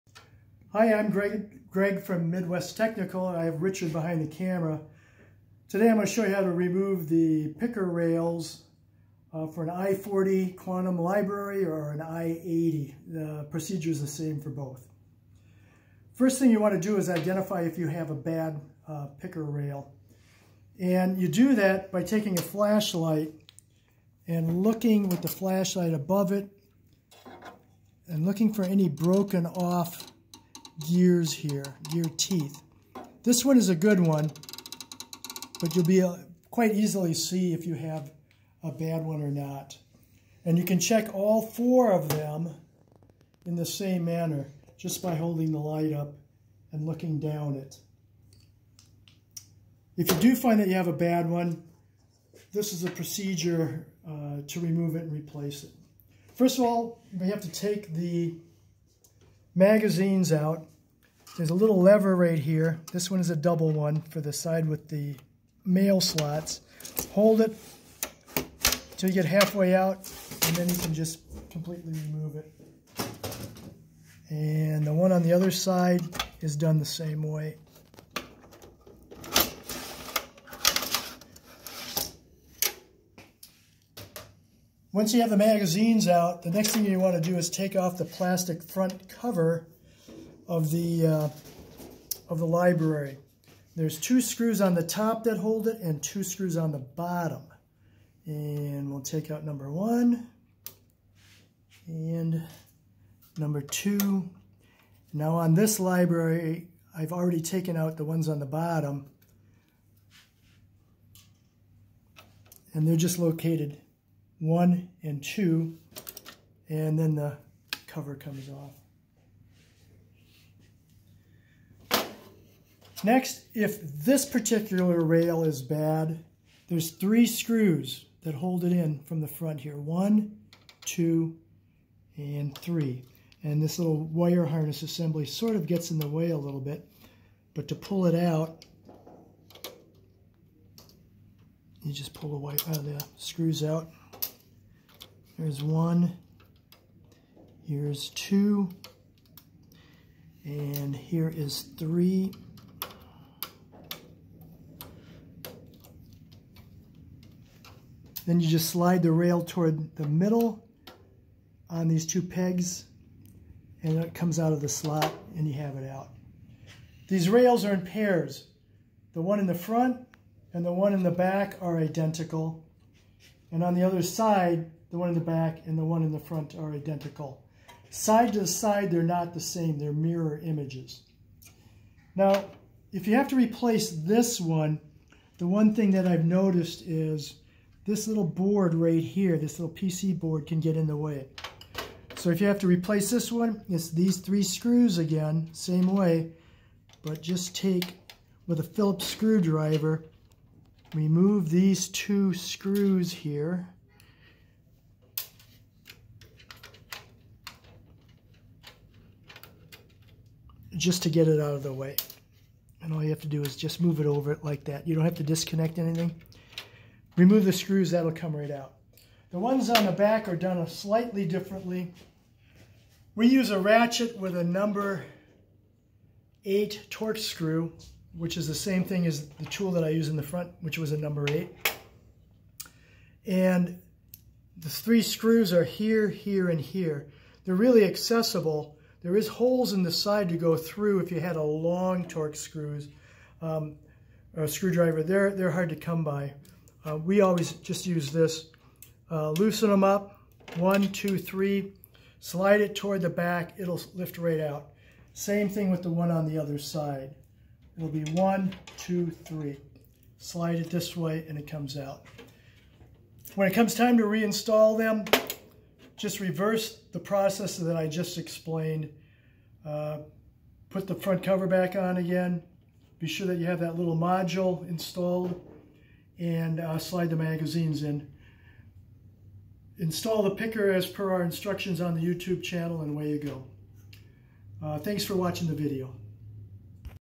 Replace Quantum i40/i80 picker gear rails
We demonstrate how to replace picker gear rails on Quantum i40 or i80 tape library.

Timestamps:
0:00 Identify Bad Picker Rails
0:55 Remove Magazines
1:30 Remove Faceplate
2:04 Remove Front Picker Rails
3:38 Remove Rear Picker Rails
4:20 Conclusion